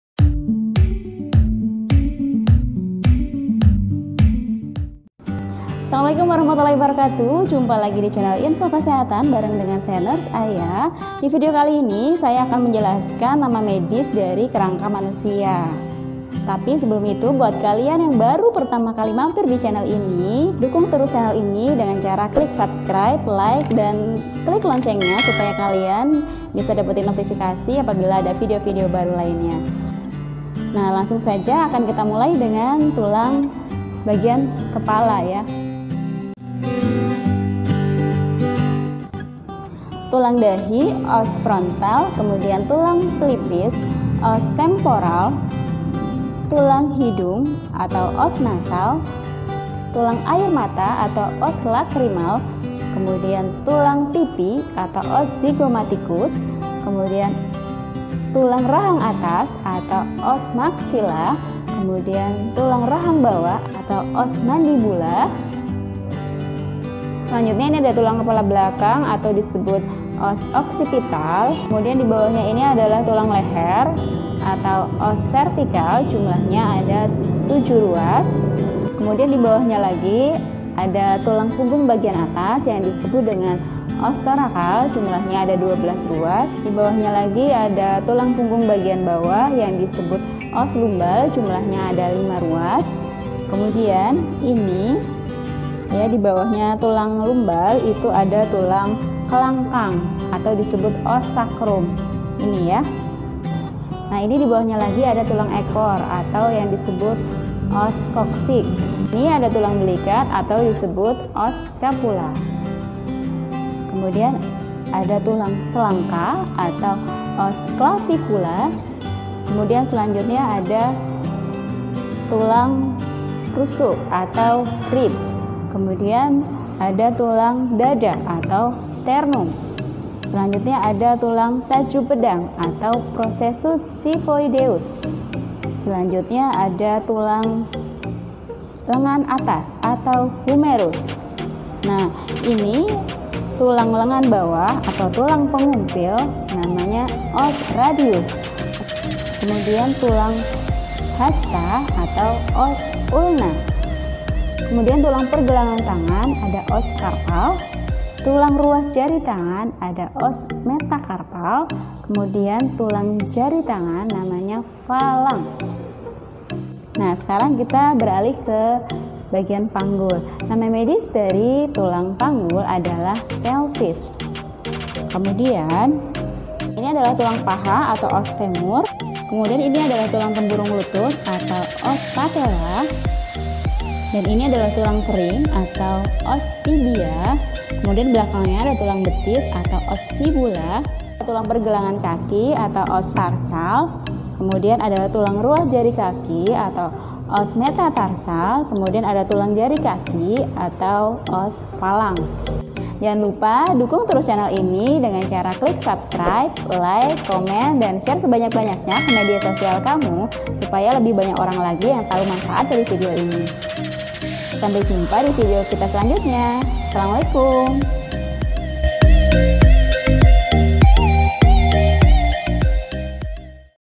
RANGKA MANUSIA LENGKAP DENGAN NAMA LATINNYA
Fungsi tulang yaitu untuk melindungi sekaligus membentuk tubuh. Tengkorak berfungsi melindungi otak sementara tulang rusuk melindungi organ vital di bagian dada. Selain itu ada lima fungsi utama lainnya dari tulang manusia, di antaranya yaitu:
1. Sebagai alat gerak pasif
2. Mendukung tubuh secara struktural untuk aksi mekanis jaringan lunak seperti kontrasi otot.
2. Tempat menyimpan mineral dan lipid yang dibutuhkan oleh tubuh.
3. Memproduksi sel darah merah, sel darah putih, dan elemen darah lainnya. Lalu, seperti apa sih anatomi tulang manusia? Simak penjelasan lengkap mengenai anatomi tulang pada manusia berikut ini.
Buat temen2 yang lagi butuh alat kesehatan ini aku kasih link toko2 di Shopee yang murah & recommended ya :
 Thanks for your support teman2...